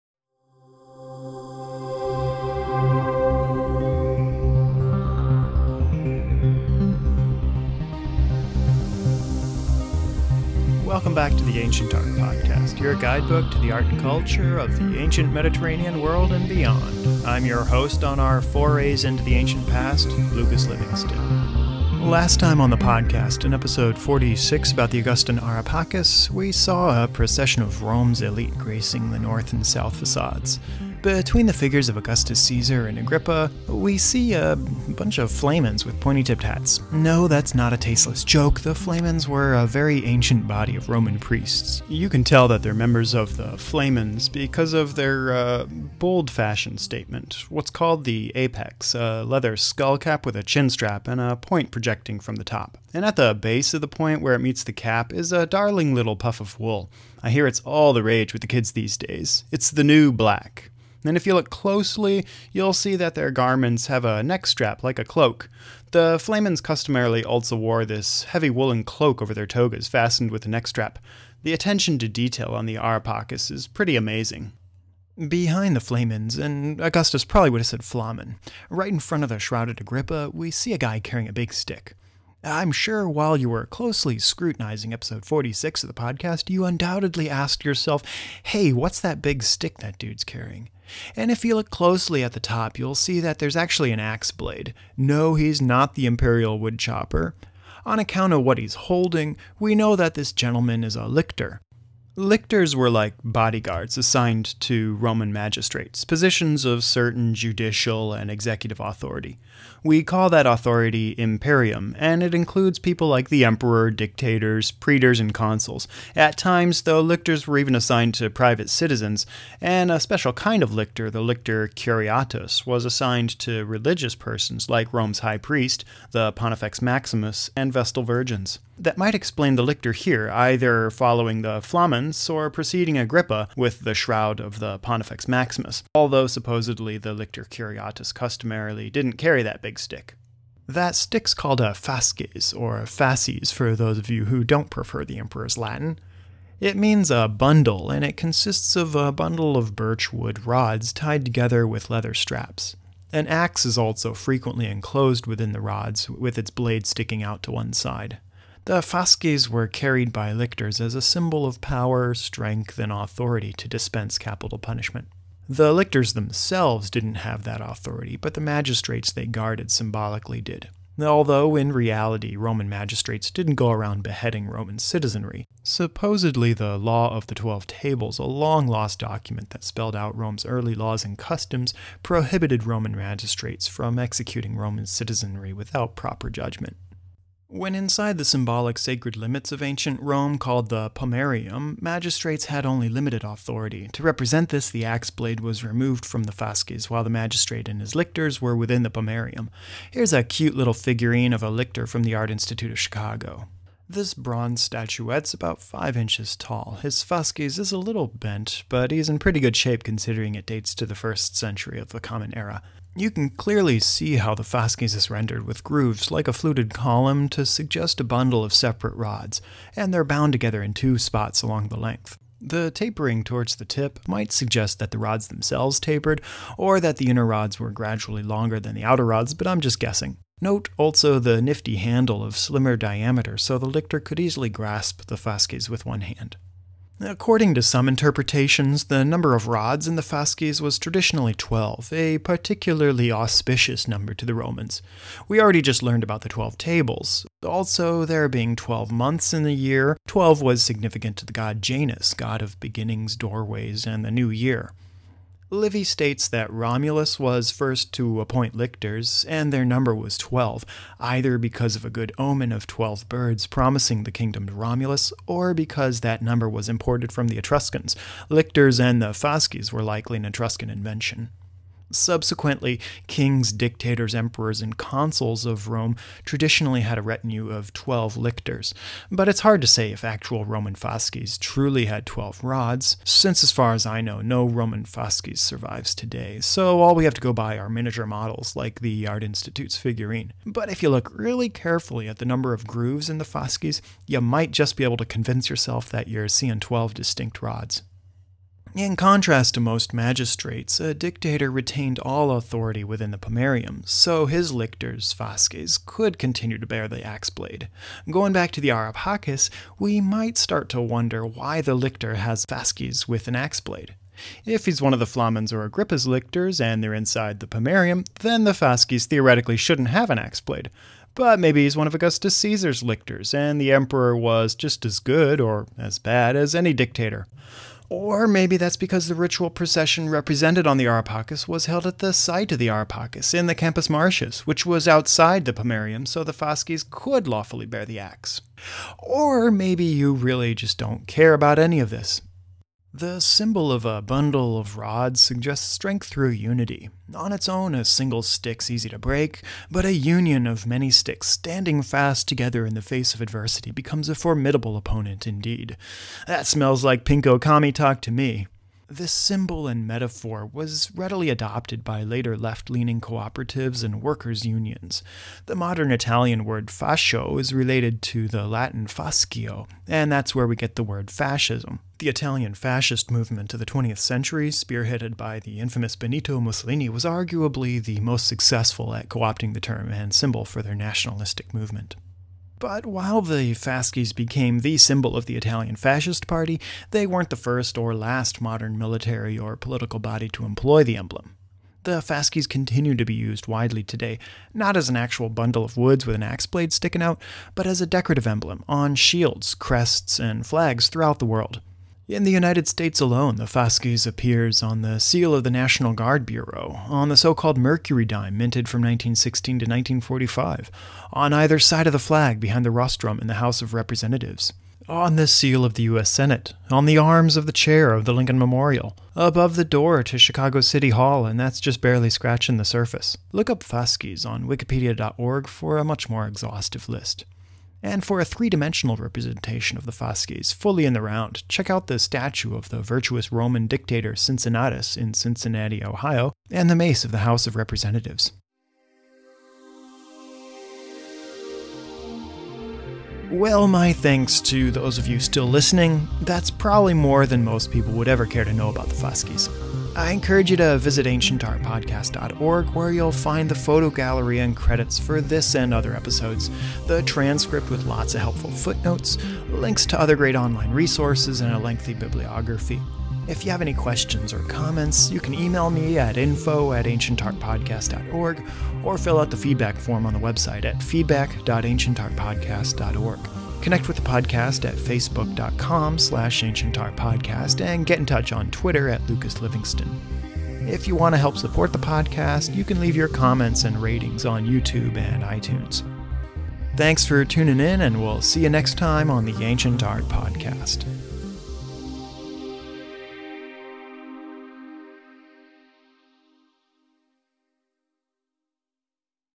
Roman Fasces (Ancient Art Podcast 47)
Just how much can we discuss about a pile of wood? Well, episode 47 of the Ancient Art Podcast about the Roman Fasces dares to find out! Composed of a bundle of rods with an axe blade sticking out, this ancient symbol of solidarity and judicial authority was a very recognizable sight in Ancient Rome. We explore the symbolism of the fasces, its different makes and models, where it came from and where it went. The fasces remains highly popular today on military and political crests, emblems, and shields across the world. We'll also be introduced to some good Roman citizens, whose careers crossed paths with the fasces, including lictors bodyguards, flamen priests, and magistrates. Explore more episodes, image galleries, credits, transcripts, and additional resources